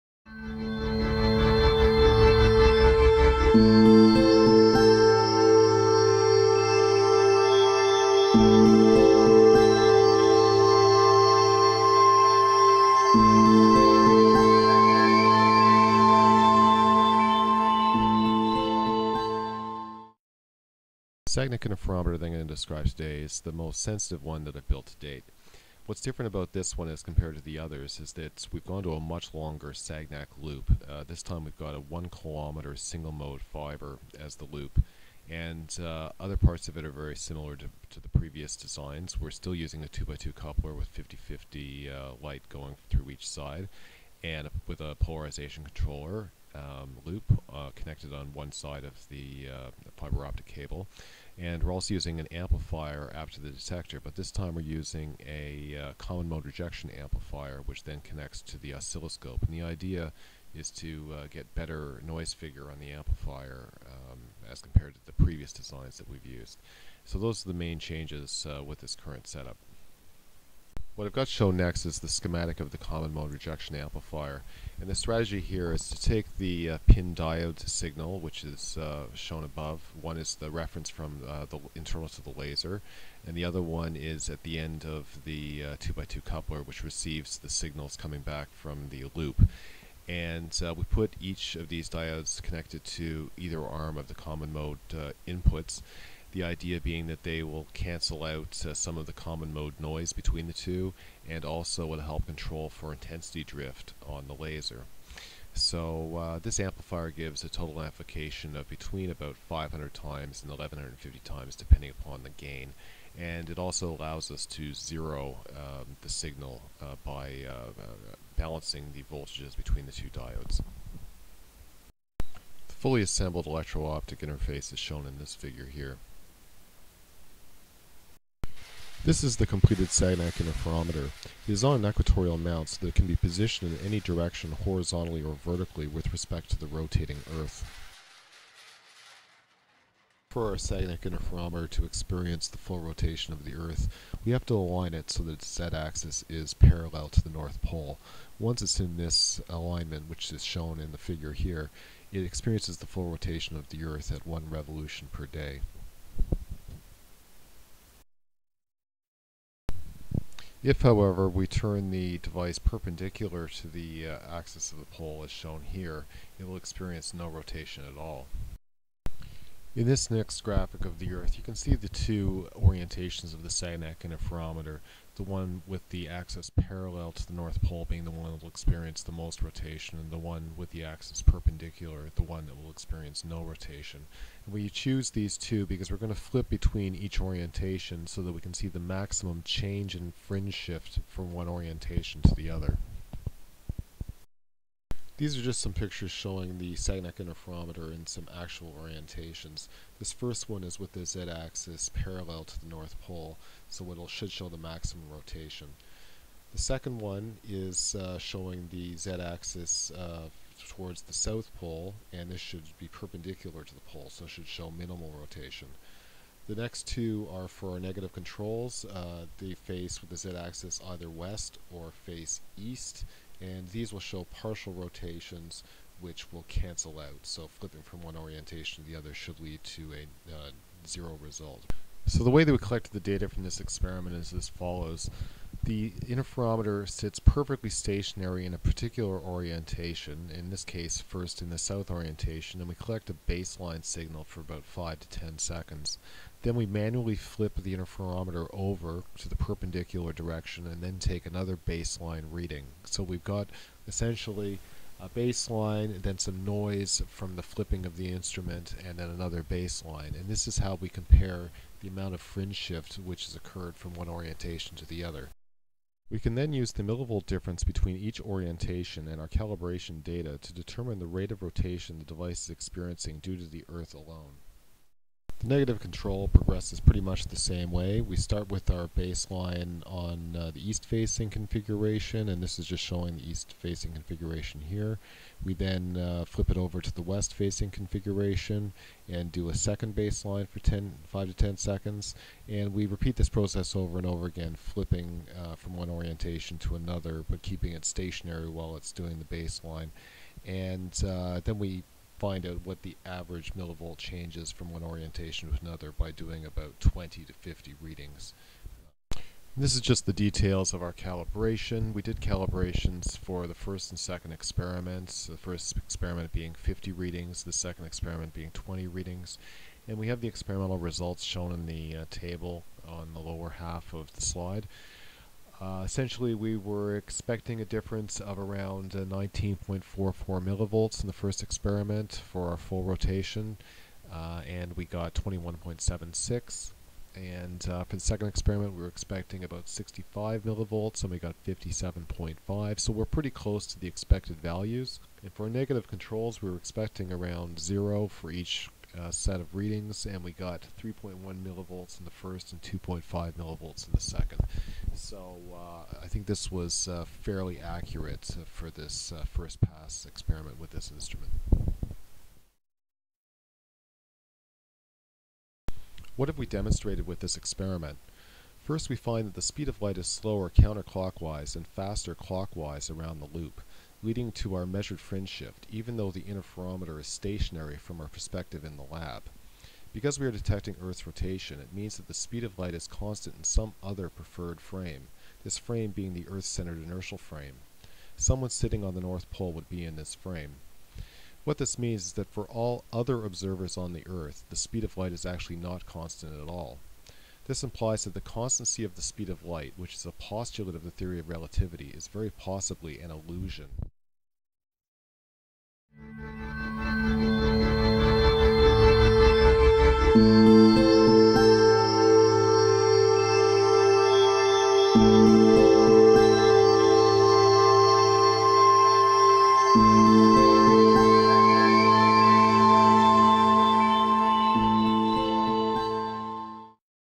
Detecting Earth's Diurnal Rotation Using a Large Area Sagnac Interferometer
This video describes the construction and testing of a Sagnac interferometer / Fiber Optic Gyroscope (FOG) sensitive enough to detect earth's rotation through space by detecting the speed of light difference in the clockwise and counter-clockwise directions from the perspective of the rotating observer. This discussion is also related to the one way speed of light question.
Fiber optic gyroscope kits are now available at skyhunt.net.